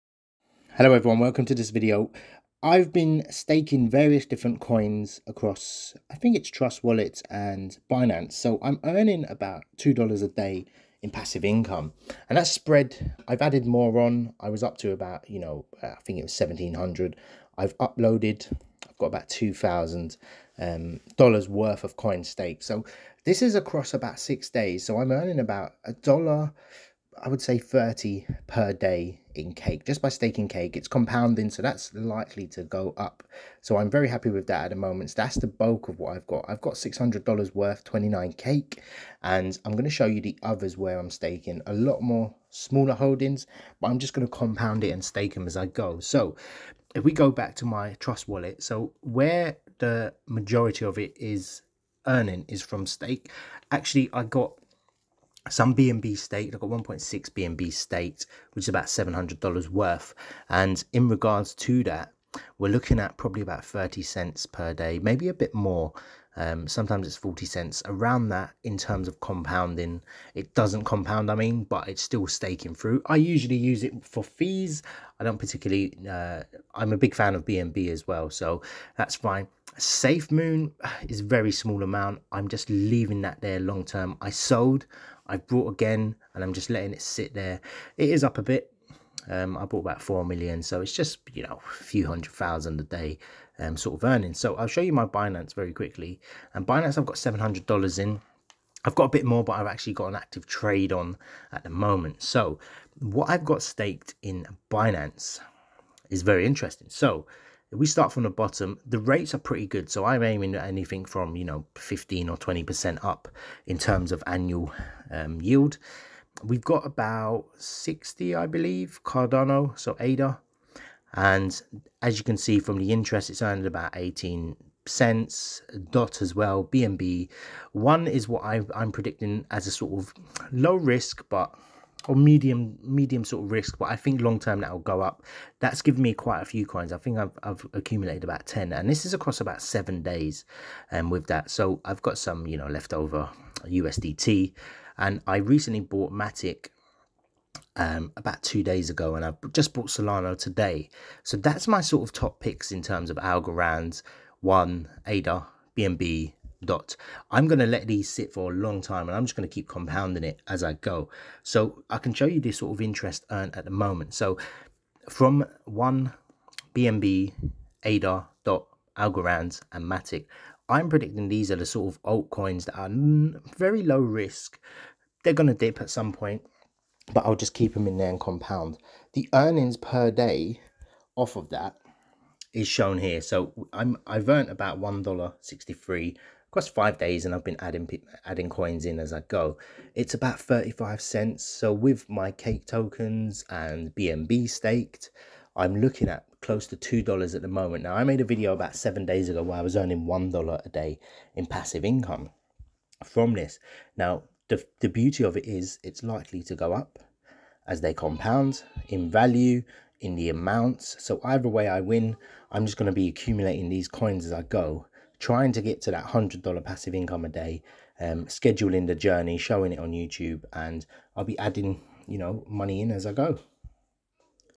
$2 Dollars a day passive income from cryptocurrency
I earn passive income by staking coins in interest on Binance, Trust wallet and pancake swap.